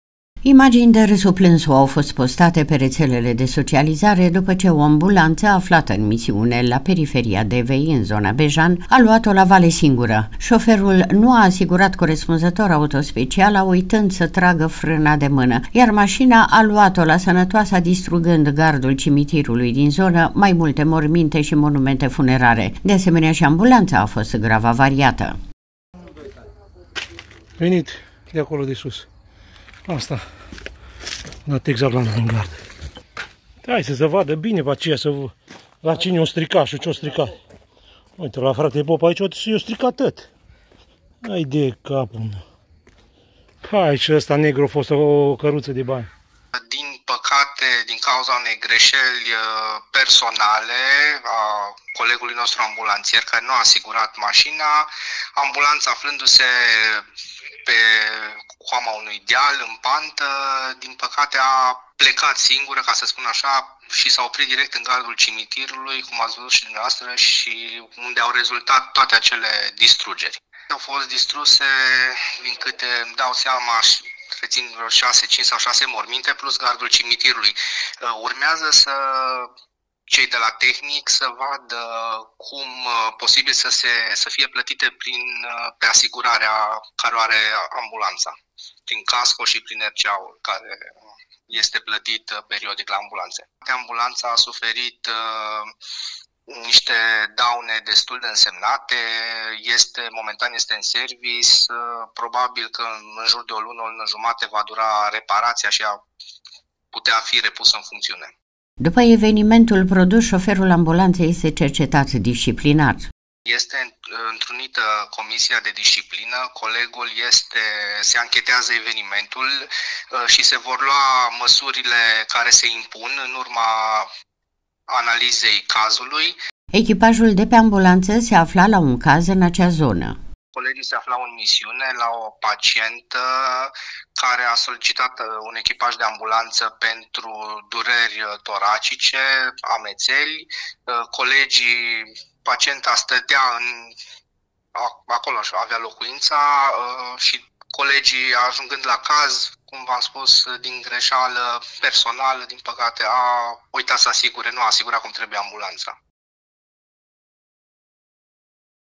O AMBULANȚĂ DIN DEVA A GREȘIT ADRESA – A AJUNS ÎN CIMITIR
AUTOSPECIALA A LUAT-O LA VALE, DISTRUGÂND GARDUL UNUI CIMITIR ȘI MAI MULTE MORMINTE
 Imagini de râsu’ - plânsu’ au fost postate pe rețelele de socializare, după ce o ambulanță, aflată în misiune, la periferia Devei, în zona Bejan, a luat-o la vale singură. Șoferul nu a asigurat corespunzător autospeciala, uitând să tragă frâna de mână, iar mașina a luat-o la sănătoasa distrugând gardul cimitirului din zonă, mai multe morminte și monumente funerare. De asemenea, și ambulanța a fost grav avariată.